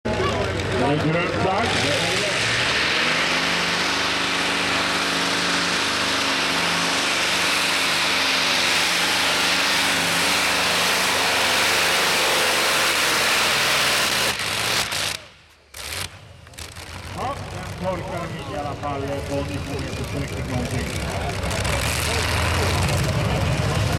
Europees Kampioenschap Tractorpulling Ekeröd 2010 : Herbie Finale Run
europees kampioenschap tractorpulling : de stermotor machine uit de Nederlandse B-divisie was ook aanwezig in zweden.. en zeer verdienstelijk rijd de machine in de finale van de 2,5 ton klasse